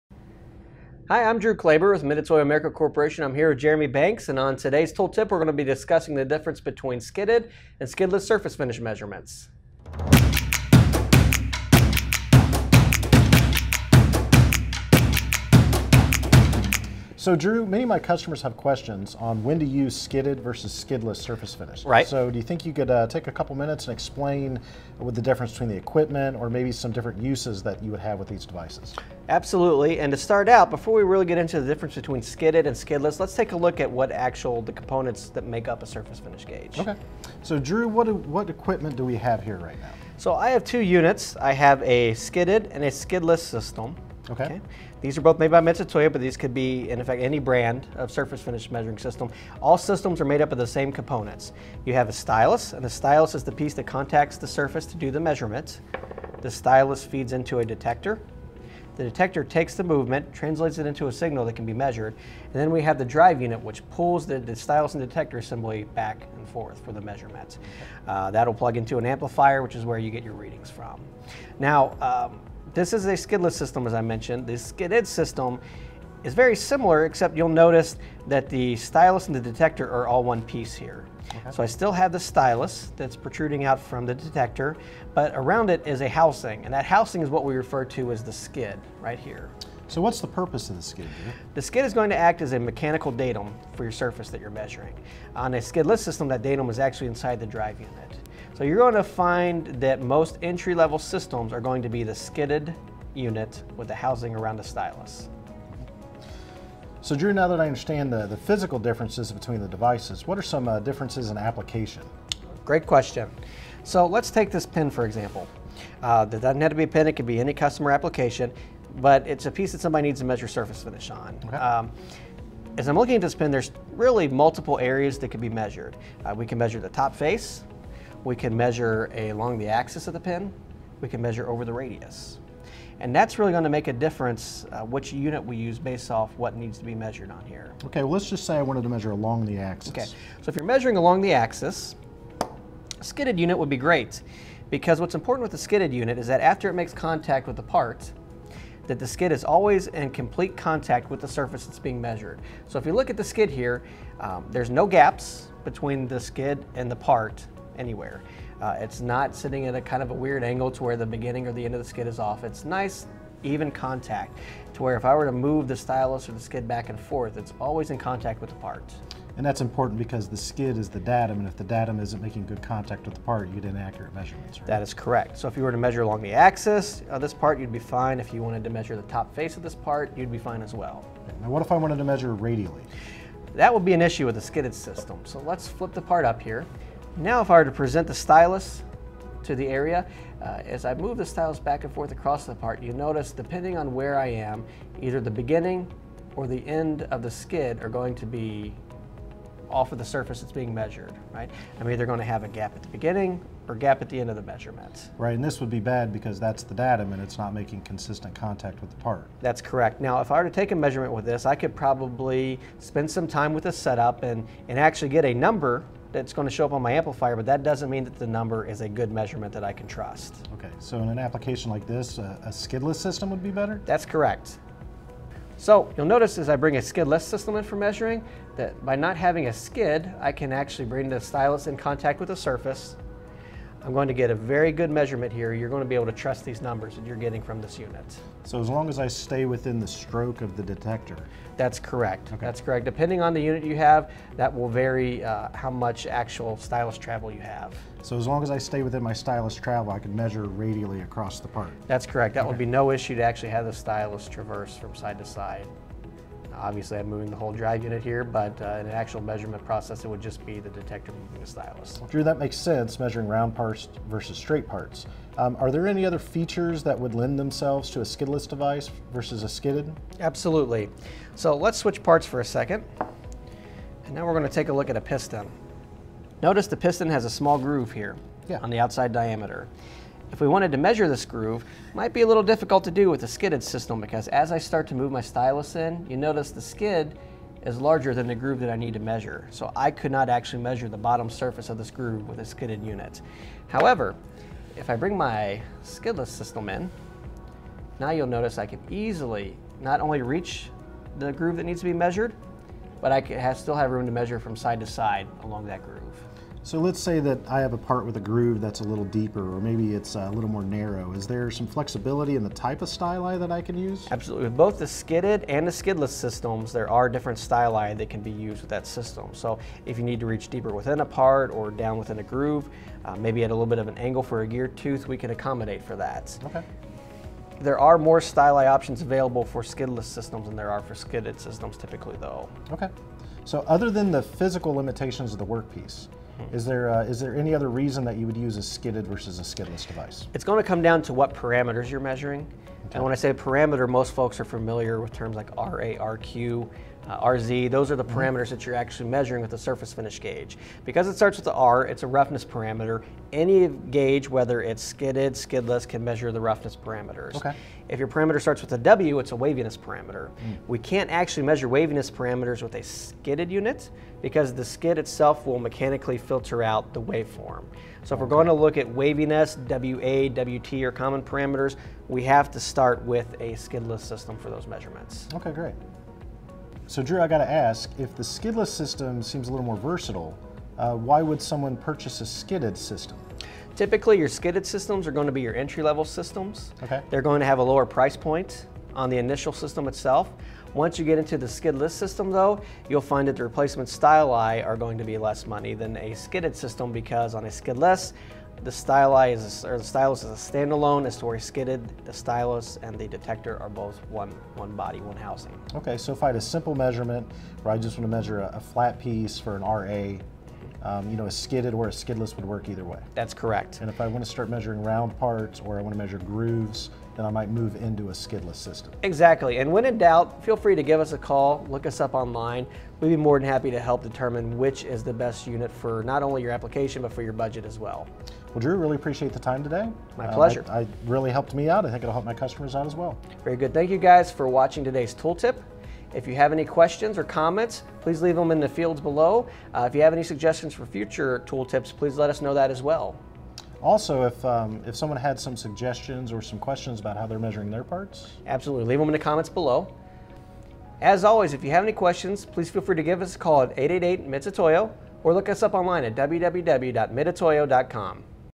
Surface Finish Measurement - Skidded VS. Skidless Surface Roughness Measurement
In this episode of Mitutoyo's Tool Tip, we look at the difference between skidded and skidless surface roughness measurement. Surface roughness measurement or surface finish, can be measured using various metrology roughness tester equipment such as the SJ-210 from Mitutoyo.

Mitutoyo Tool Tip 013 Skidded VS Skidless